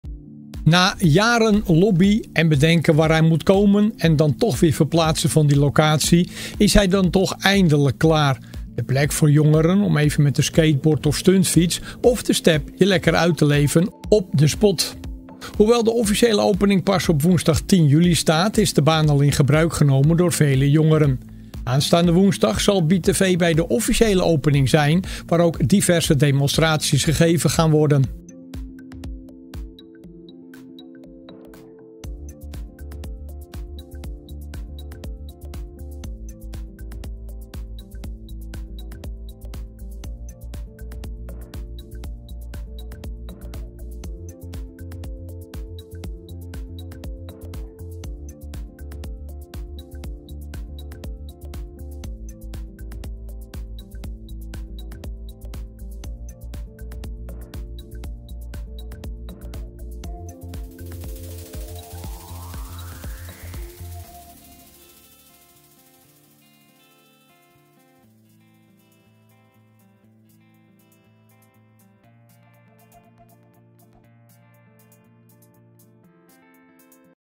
The Spot in Heiloo wordt al gebruikt door de jeugd (7 juli 2024)
Na jaren lobby en bedenken waar hij moet komen en weer verplaatsen van die locatie, is hij dan toch eindelijk klaar. De plek voor jongeren om even met de skateboard of stuntfiets of step je lekker uit te leven op The Spot.